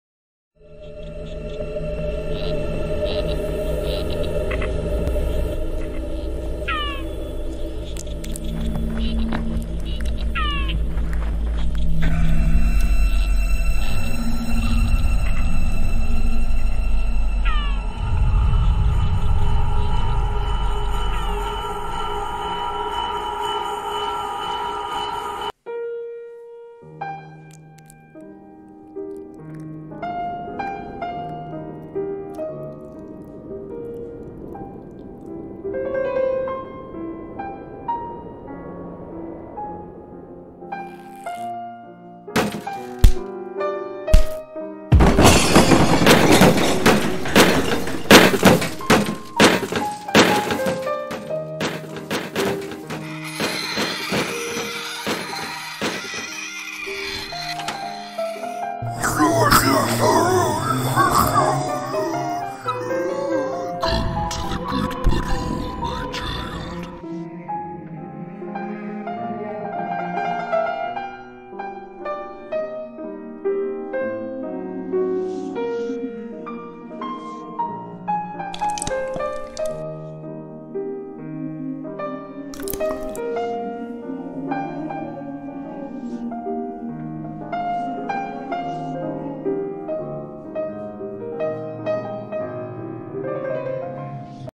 Birth - A Battle Between Holes
A New Father finds that he is the key to the good hole but must fight off the hand of the evil hole.